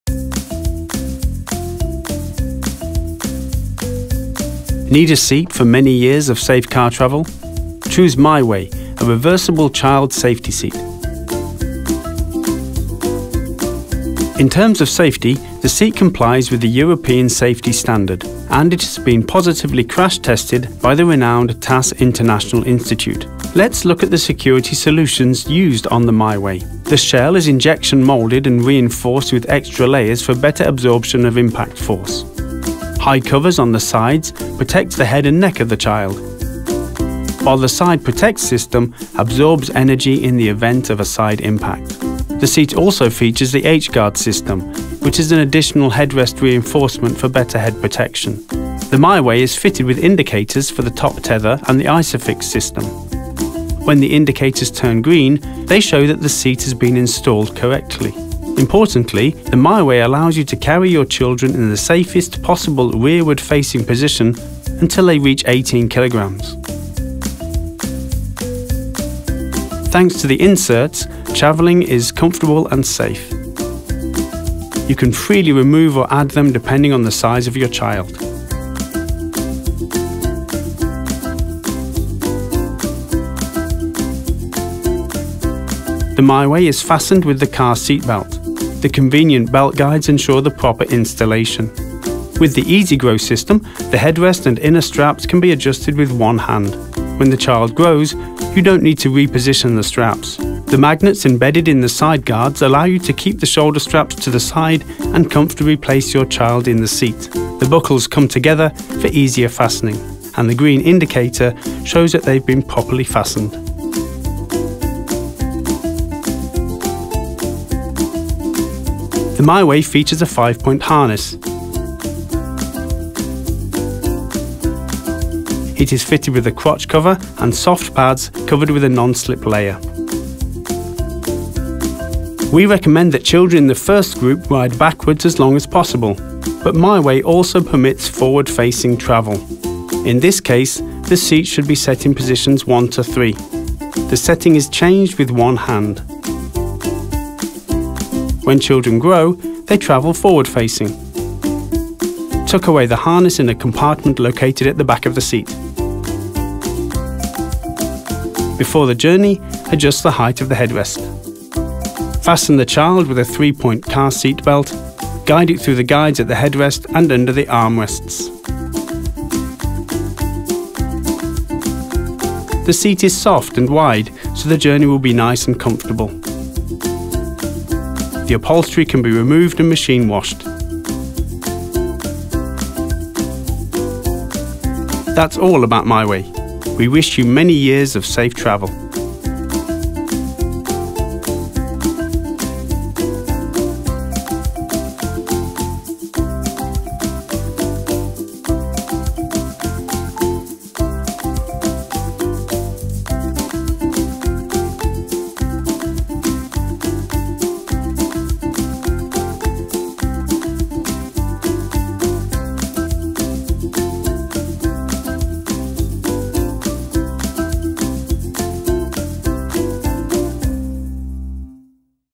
Kinderkraft MYWAY 0-36 kg car seat | ISOFIX | RWF 0-18 kg
Learn all about the MYWAY 0-36 kg car seat by Kinderkraft – this is a car seat in the ECE 0/I/II/III group and is suitable for rearward facing travel by children up to 18 kg! MYWAY is installed using ISOFIX, the TOP TETHER strap and the car's seat belts (depending on the age group). The car seat is mounted on a sturdy base that increases your child's safety by absorbing the first force of impact in the event of a collision. MYWAY has three indicators of correct installation to help you install it correctly in your car. It's equipped with an adjustable five-point safety harness with soft pads and a crotch cover.

It has four seat positions: one rearward facing (RWF) and three forward facing (FWF). The seat can be adjusted using one hand. The seat itself is soft and comfortable, and the cover is pleasant to the touch and made of materials that are easy to clean. It comes with three inserts for young children to increase not only your child's safety, but also comfort during travel. It features a head restraint with 10 positions (adjusts using one hand) and magnetic clips for the safety harness to make it easier for you to place your child inside the car seat.

The MYWAY car seat by Kinderkraft is equipped with special systems to improve safety and comfort for your child:

- SIDE PROTECT SYSTEM: the shell of the car seat is reinforced, meaning it absorbs the impact of side collisions.

- H-GUARD SYSTEM: the head restraint features three layers thanks to which it better protects the child's head and neck region of the spine.

- EASY GROW SYSTEM: enables quick and simultaneous adjustment of the head restraint and safety harness without needing to switch anything around.

- RECLINING SYSTEM: allows the angle of incline of the car seat to be adjusted using one hand.

- Anti-Slippery System: the safety harness pads feature non-slip strips so they don't slide off your little one's shoulders.

In addition, the MYWAY car seat features belt guides that make it easier to correctly secure your child with the car's three-point seat belt (forward facing optional from 9 kg).

The Kinderkraft MYWAY car seat came first in the Mother & Baby Awards 2021, and was also recognised by the independent German magazine ALLES BESTE.